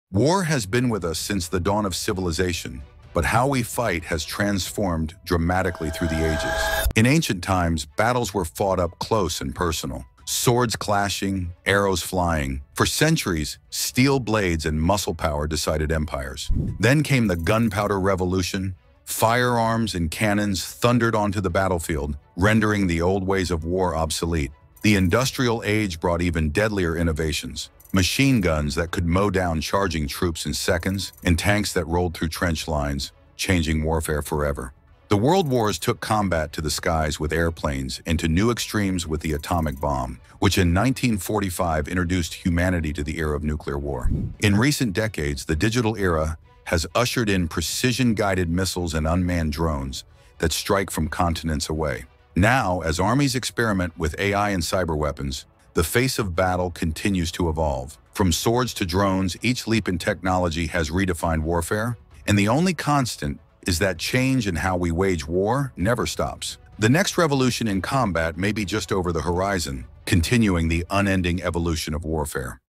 From Swords to Drones: The Evolution of Warfare
War has been with us since the dawn of civilization, but how we fight has transformed dramatically through the ages. In ancient times, battles were fought up close and personal – swords clashing, arrows
flying. For centuries, steel blades and muscle power decided empires. Then came the gunpowder revolution: firearms and cannons thundered onto the battlefield, rendering the old ways of war obsolete. The industrial age brought even deadlier innovations – machine guns that could mow down charging troops in seconds, and tanks that rolled through trench lines, changing warfare forever. The world wars took combat to the skies with airplanes and to new extremes with the atomic bomb, which in 1945 introduced humanity to the era of nuclear war. In recent decades, the digital era has ushered in precision-guided missiles and unmanned drones that strike from continents away. Now, as armies experiment with AI and cyber weapons, the face of battle continues to evolve. From swords to drones, each leap in technology has redefined warfare – and the only constant is that change in how we wage war never stops. The next revolution in combat may be just over the horizon, continuing the unending evolution of warfare.